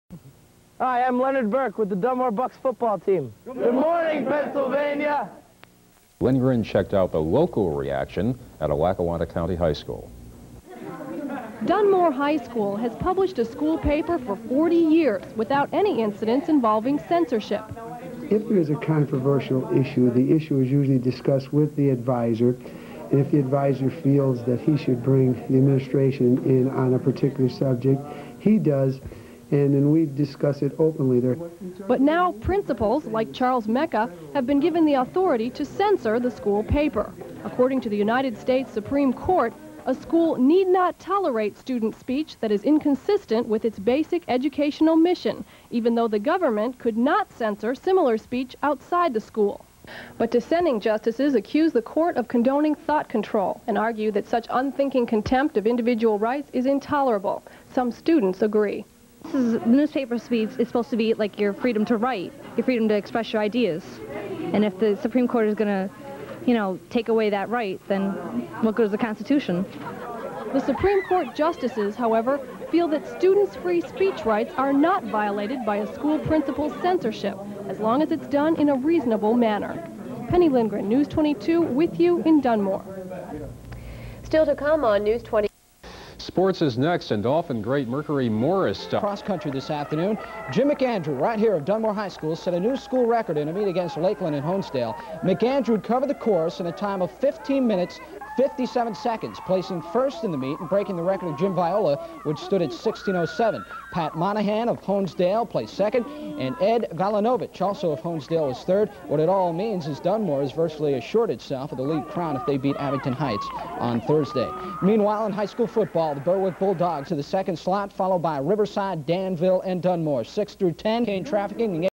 3 Dunmore High School Clips: 87 Bucks ; Newspaper Censor; Jim McAndrew breaks Record
3 Dunmore Clips I found on my VHS tape from 1987-88 !

1. Lenny Burke of The 1987 Dunmore Bucks saying "GOOD MORNING PENNSYLVANIA." Sept. 1987

2. WYOU TV-22's Penny Lindgren reports on censoring school newspapers at Dunmore High, Fall 1987. This is the DHS Class of 1988 in this video.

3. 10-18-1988 Jim McAndrew (Macker) breaks the Dunmore High School Cross Country record at 15:57 Min, breaking previous record held by Jim Viola at 16:07.